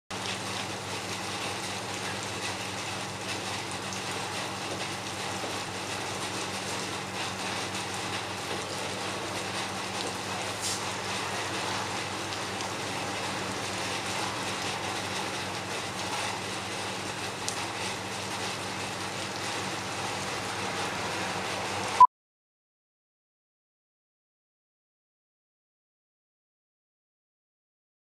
Rain 03 / Free Stock Footage
Description:
Footage rain dripping from a roof. Water pools during heavy rain. Rain water dripping from the ceiling.